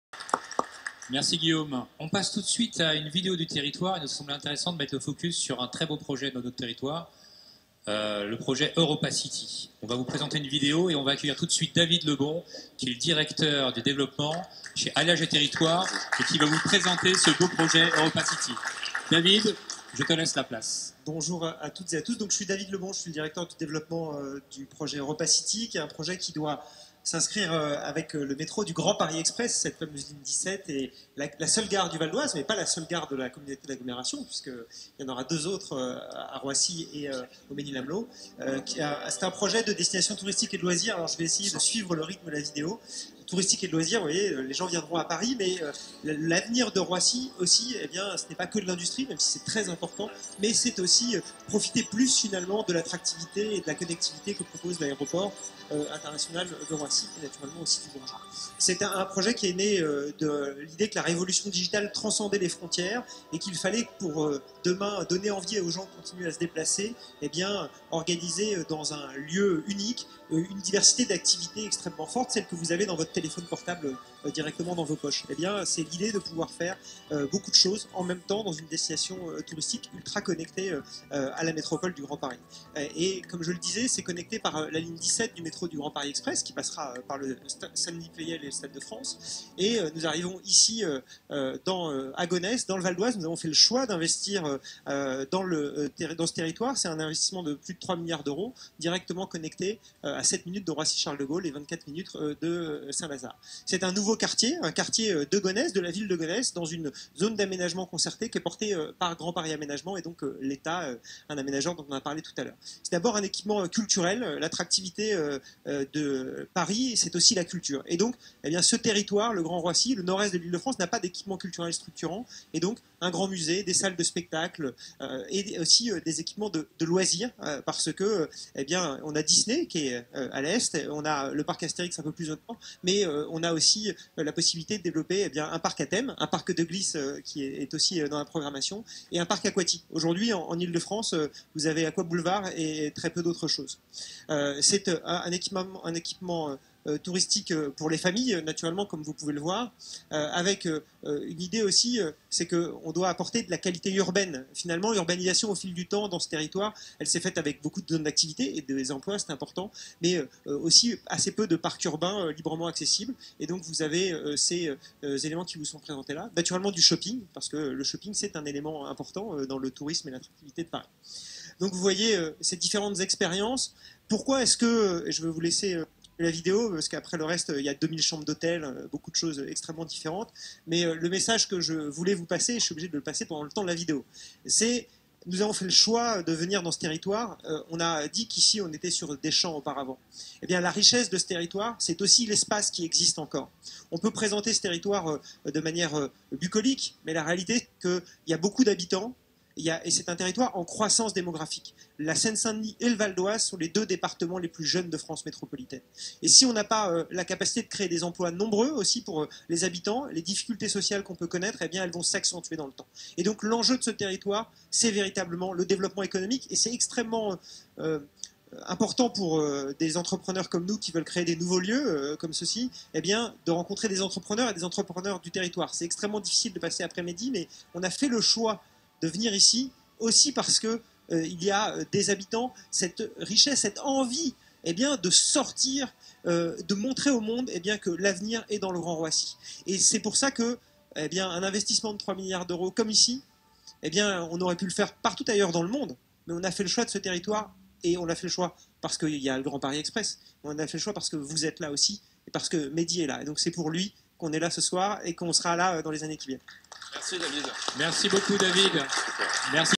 "Pourquoi EuropaCity a fait le choix d'investir plus de 3 milliards d'€uros dans le Grand Roissy ?"
Notre réponse lors de la présentation du projet devant les principaux acteurs de Roissy Dév Aérotropolis et Roissy Entreprises  - "On a fait le choix de venir investir plus de 3 milliards d'€uros dans ce territoire, pour ses habitants, avec cette envie de montrer au monde que l’avenir est dans le Grand Roissy" - David Lebon, directeur du Développement EuropaCity.

"EuropaCity est un projet de destination touristique et de loisir. L’avenir de Roissy ce n’est pas que de l’industrie, c’est aussi profiter plus de l’attractivité et de la connectivité que propose l’aéroport international de Roissy et naturellement aussi celui du Bourget.
C’est un projet qui est né de l’idée que la révolution digitale transcendait les frontières, et qu’il fallait pour demain donner envie aux gens de continuer à se déplacer, organiser dans un lieu unique une diversité d’activités extrêmement fortes, celle que vous avez dans votre téléphone portable directement dans vos poches, c’est l’idée de pouvoir faire beaucoup de choses en même temps dans une destination touristique ultra connectée a la métropole du Grand Paris. Comme je le disais c’est connecté par la ligne 17 du Grand Paris Express qui passera par Saint Denis Pleyel et le stade de France, et nous arrivons ici à Gonesse dans le Val-d’Oise. Nous avons fait le choix d’investir dans ce territoire, c’est un investissement de plus de 3 milliards d’euros directement connecté à 7min de Roissy CDG et 24 min de St Lazare. C’est un nouveau quartier de Gonesse dans une zone d’aménagement concertée portée par GPA et l'Etat. C’est d’abord un équipement culturel, l’attractivité de Paris c'est aussi la culture, et bien ce territoire, le Grand Roissy, le nord-est d’ile de France n’a pas d’équipement culturel structurant et donc un grand musée, des salles de spectacle, et aussi des équipements de loisir parce que on a Disney qui est a l’est, on a le parc Astérix un peu plus au nord, mais on a aussi la possibilité de développer un parc a thème, un parc de glisse qui est aussi dans la programmation, et aussi un parc aquatique. Aujourd’hui en IDF, vous avez Aquaboulevard et très peu d’autres choses. C’est un équipement touristique pour les familles naturellement comme vous pouvez le voir, avec l’idée aussi, c’est que on doit apporter de la qualité urbaine, finalement l’urbanisation au fil du temps dans ce territoire elle s’est faite avec beaucoup de zones d’activités et des emplois, c’est important, mais aussi assez peu de parcs urbains librement accessibles, et donc vous avez ces éléments qui vous sont présentés là. Naturellement du shopping parce que le shopping est un élément important dans le tourisme et l’attractivité de Paris.
Le message que je voulais vous passer c’est que nous avons fait le choix de venir dans ce territoire, on a dit qu’ici on était sur des champs auparavant. Et bien la richesse de ce territoire, c’est aussi l’espace qui existe encore. On peut présenter ce territoire de manière bucolique mais la réalité c’est qu’il y a bcp d’habitants, et c’est un territoire en croissance démographique, la seine St Denis et le Val d’Oise sont les deux départements les plus jeunes de France métropolitaine. Et si on n’a pas la capacité de créer des emplois nombreux aussi pour les habitants, les difficultés sociales qu’on peut connaitre elles vont s’accentuer dans le temps. Et donc l’enjeu de ce territoire c’est véritablement le développement économique et c’est extrêmement important pour des entrepreneurs comme nous qui veulent créer des nouveaux lieux comme ceci, eh bien de rencontrer des entrepreneurs du territoire, c’est extrêmement difficile de passer après Mehdi mais on a fait le choix de venir ici aussi parce qu’il y a des habitants, cette richesse, cette envie de sortir, de montrer au monde que l’avenir est dans le grand Roissy. Et c’est pour ça qu’un investissement de 3 milliards d’euros comme ici, et bien on aurait pu le faire partout ailleurs dans le monde, mais on a fait le choix de ce territoire, et on a fait ce choix - parce qu’il y a le Grand Paris express, - mais on a fait ce choix parce que vous êtes la aussi et parce que Mehdi est là et donc c’est pour lui qu’on est la ce soir et qu’on sera la dans les années qui viennent."